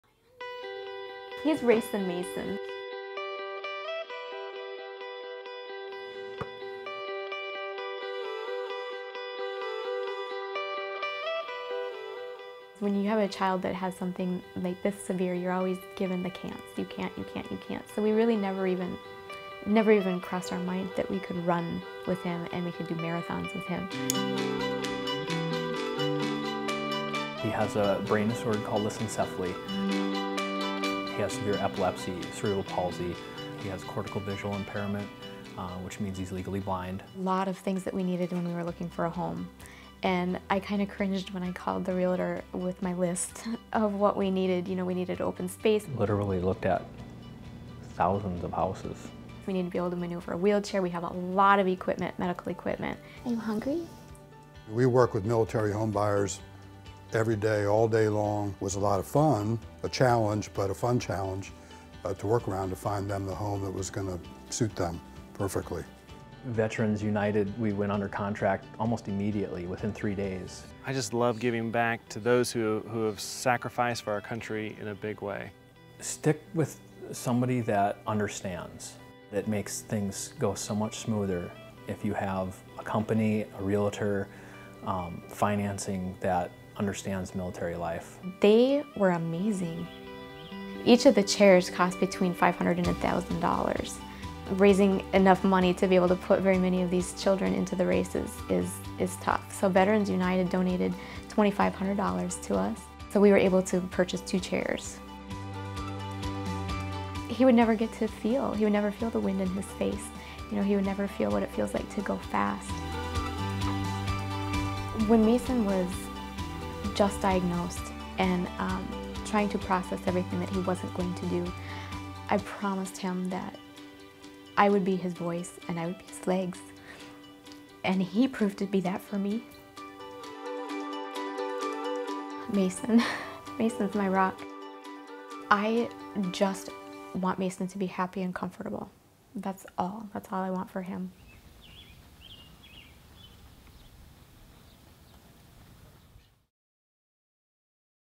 Wheelchair Racer Gets New Wheels
Mason Robinson received a new wheelchair so that he can compete in wheelchair races with his family.